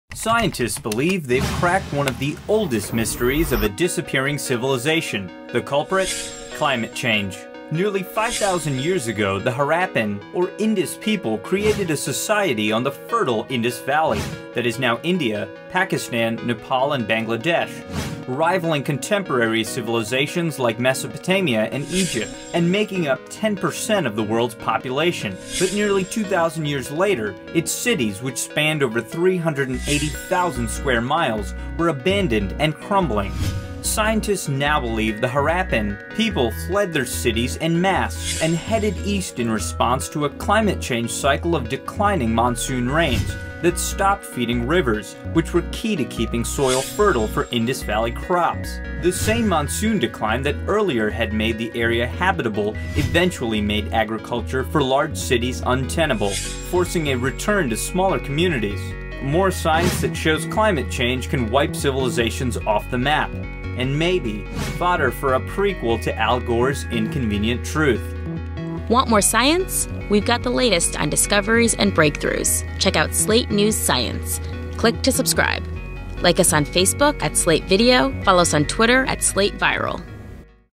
Natural Climate Change Wiped the Harappan Civilization off the Map
Follow: @SlateViral, Facebook.com/SlateVideo
Watch: YouTube.com/Slatester, SlateV.com

Scientists believe they've cracked one of the oldest mysteries of a disappearing civilization. The culprit? Climate change.

Nearly 5,000 years ago the Harappan or Indus people created a society on the fertile Indus Valley that is now India, Pakistan, Nepal, and Bangladesh—rivaling contemporary civilizations like Mesopotamia and Egypt, and making up 10 percent of the world's population. But nearly 2,000 years later its cities that spanned over 380,000 square miles were abandoned and crumbling.

Scientists now believe the Harappan people fled their cities en masse and headed east in response to a natural climate change cycle of declining monsoon rains that stopped feeding rivers, which were key to keeping soil fertile for Indus Valley crops. The same monsoon decline that earlier had made the area habitable, eventually made agriculture for large cities untenable, forcing a return to smaller communities.

More science that proves climate change can wipe civilizations off the map.